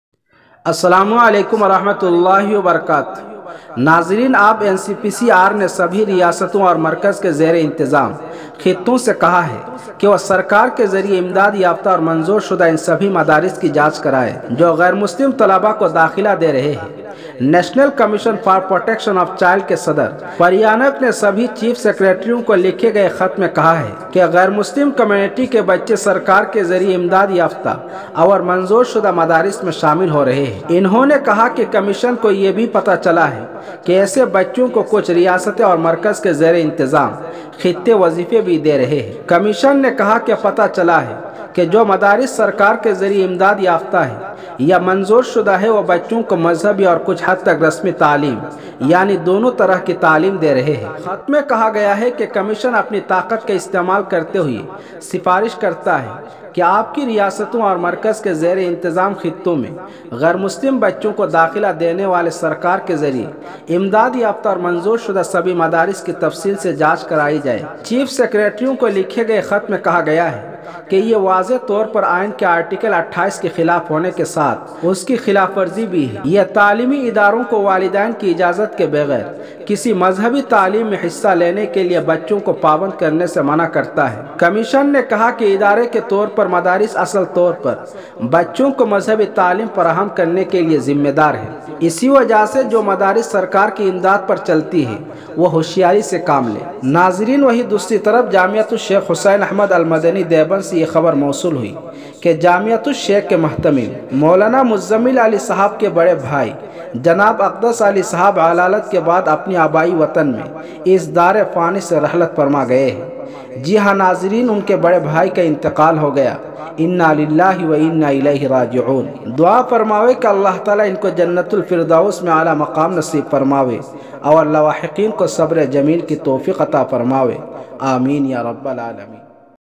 December 10, 2022 سبھی ریاست کے مدارس میں جانچ کرانے کا حکم جامعہ توشیخ کے مہتمم کو صدمہ 😢
سبھی ریاست کے مدارس میں جانچ کرانے کا حکم جامعہ توشیخ کے مہتمم کو صدمہ
sarkari madrassa sarye
light of islam